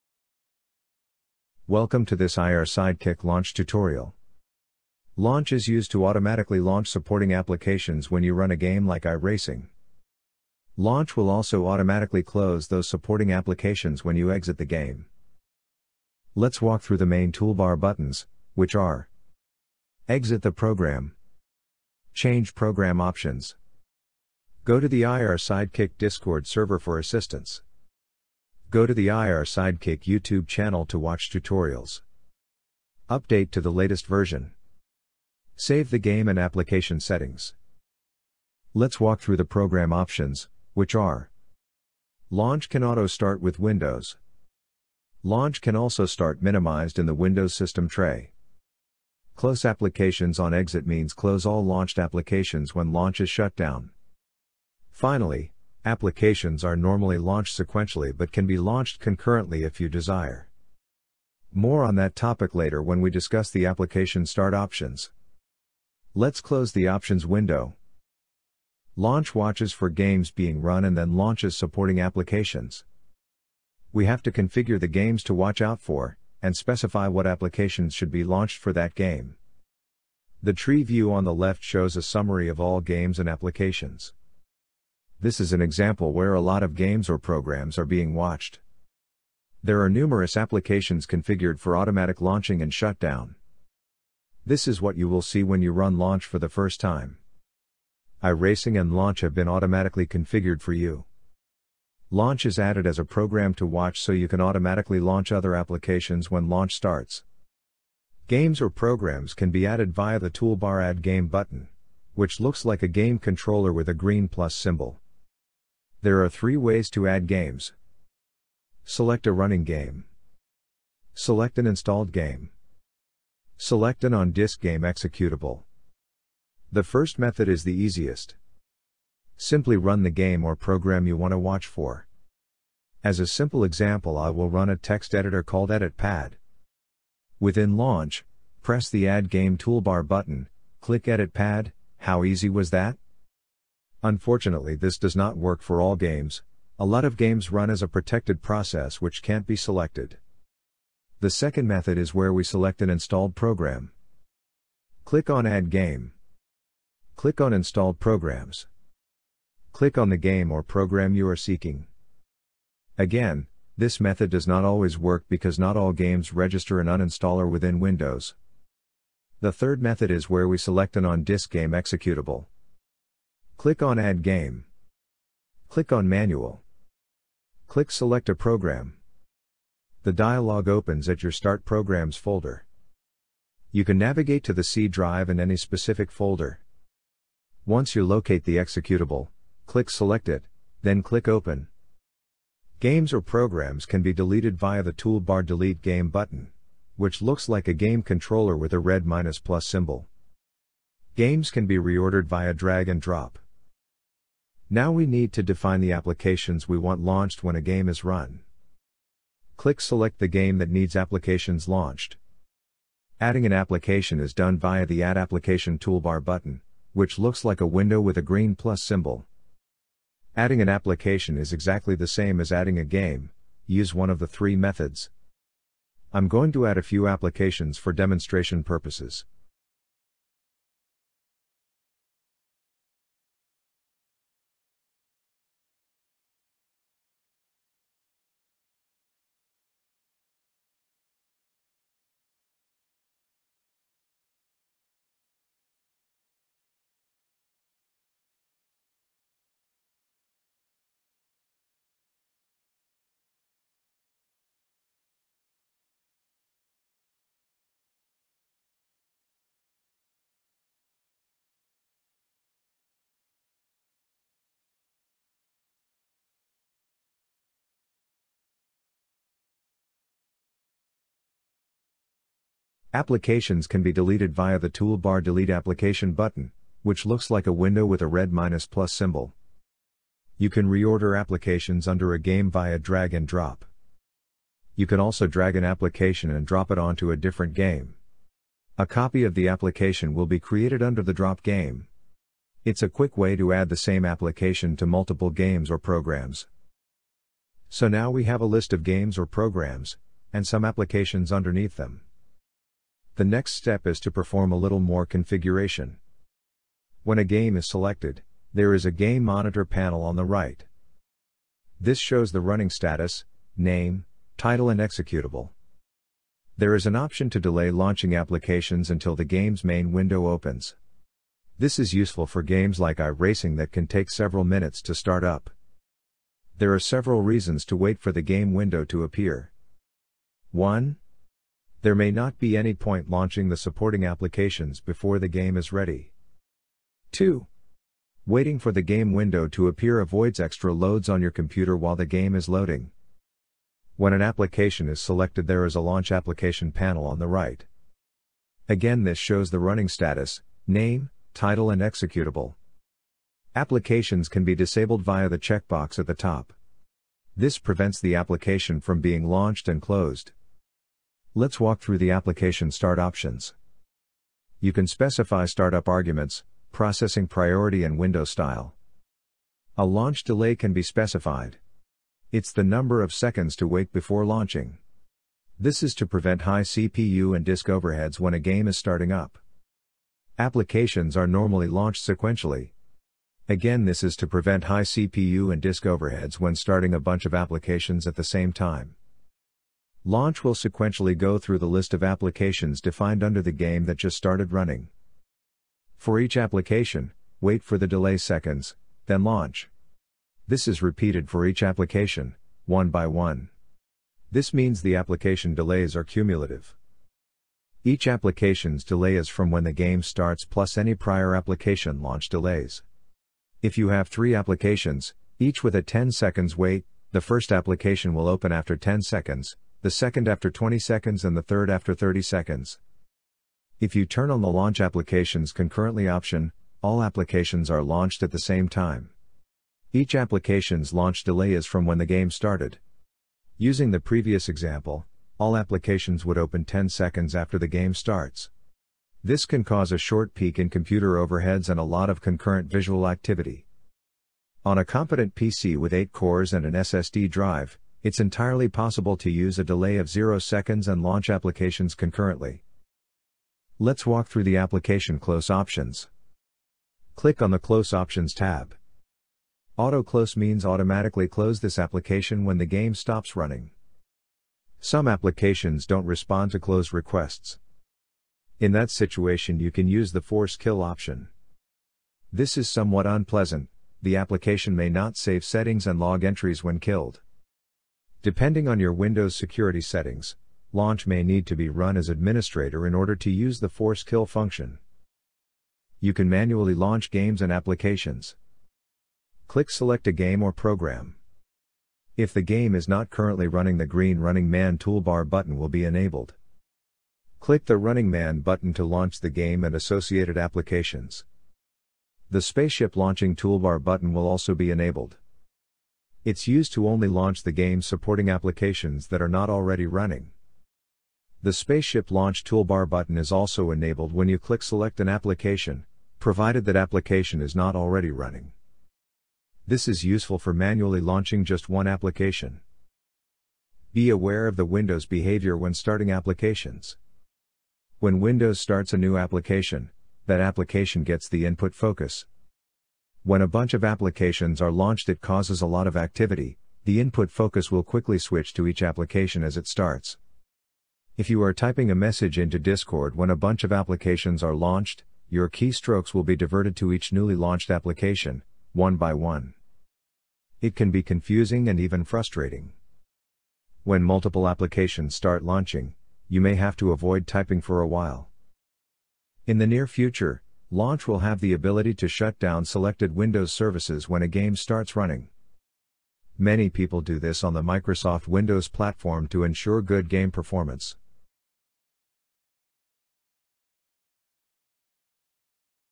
irSidekick Launch - Tutorial
What the program does and how to use it

00:00 - Intro
00:15 - Main toolbar buttons
00:37 - Program Options
01:08 - Add Games
03:23 - Add Applications
04:56 - Game configuration
05:43 - Application Configuration
06:00 - Application Start Options
06:19 - Sequential/Concurrent Launching
07:32 - Application CLose Options
08:04 - Manually launch games/apps
08:42 - Windows Behaviour
09:18 - More features coming